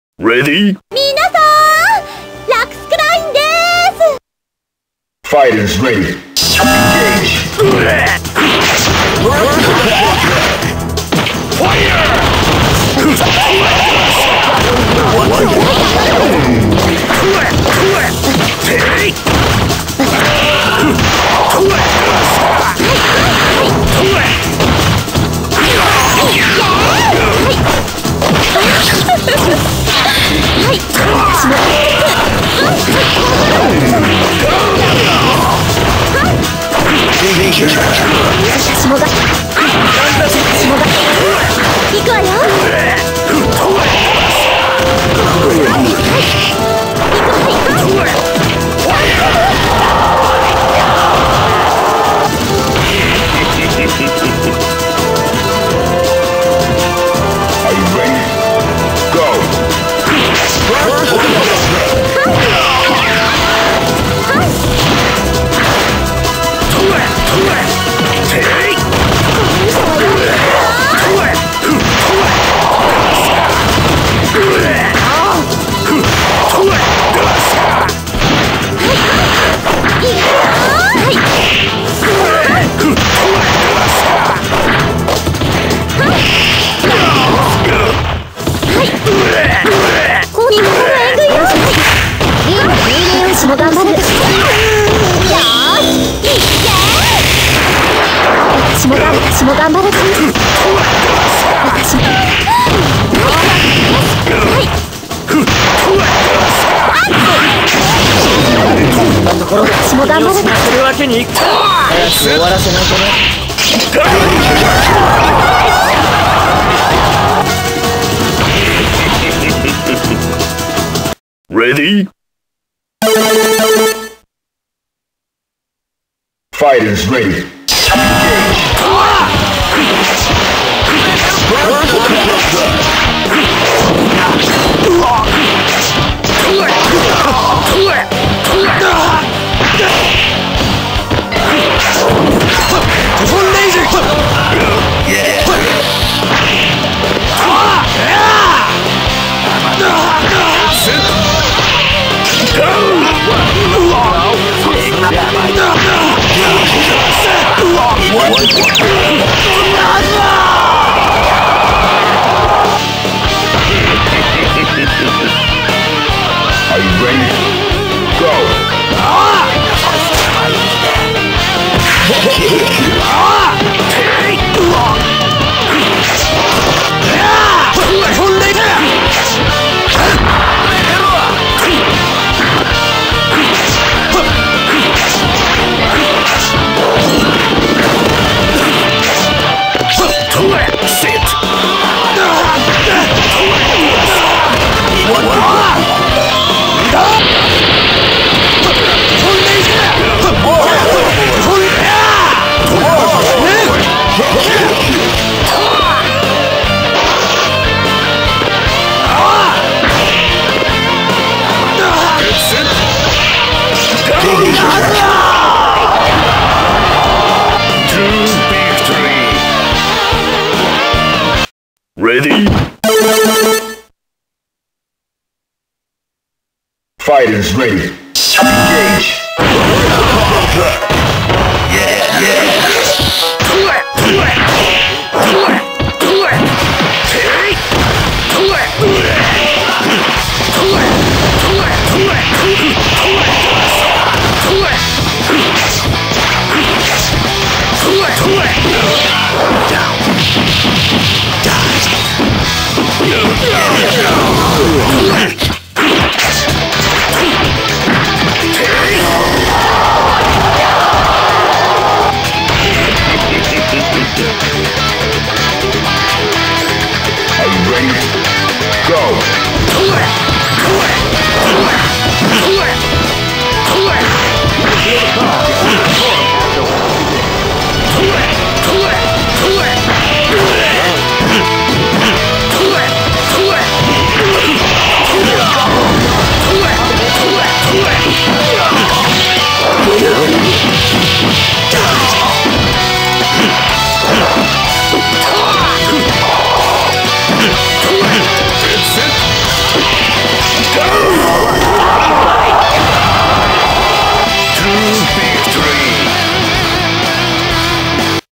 ROLENTO RAEG
Just me using Rolento to fight 3 characters and him letting out steam after he watched Gundam SEED destiny.
Stage1: Hangar Base by: Flamekyo